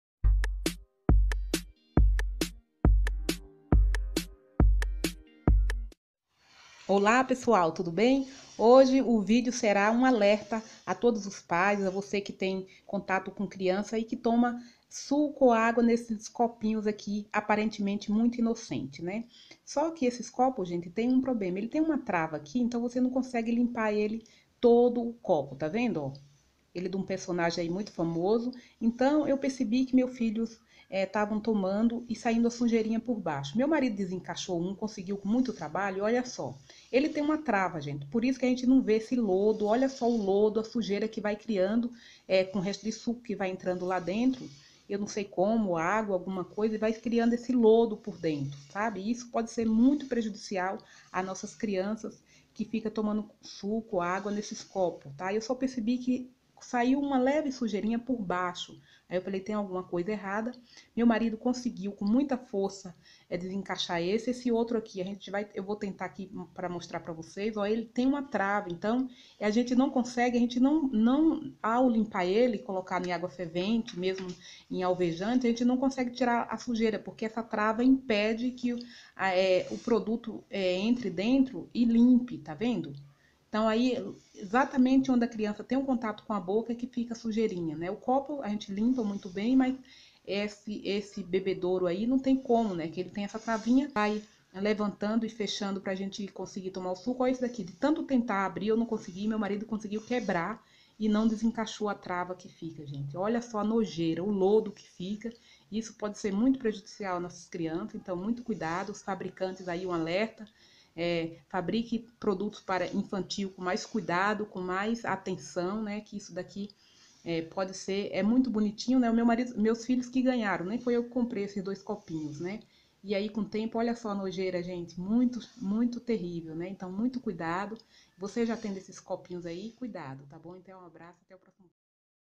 Alerta! Cuidado c/ Esses Copos Infantis✌😫resenha.
Olha a sujeira nesse inocente copo infantil 🙈🙊💩💩. Essas garrafinhas que usamos para nossos filhos beber água ou suco, pode acumular um nojento lodo e prejudicar nossas crianças. Fica o alerta.

🙋👉🙇Divulguem o vídeo para seus amigos :)💯